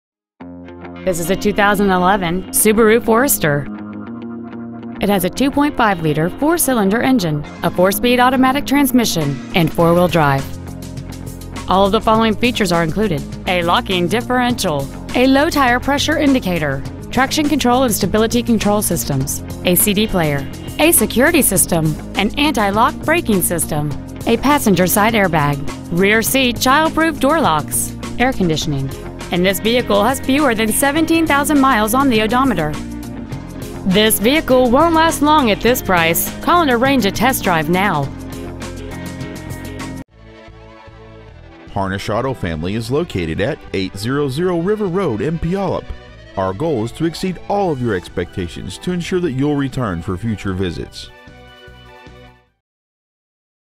2011 Subaru Forester Puyallup WA
Year : 2011
 Make : Subaru
 Model : Forester
 Engine : 2.5L 4Cyl
 Trans . : 4 Spd Automatic
 Exterior : Marine Blue Pearl
 Miles : 16,121

 Stock : U3560

 2011 Subaru Forester Puyallup WA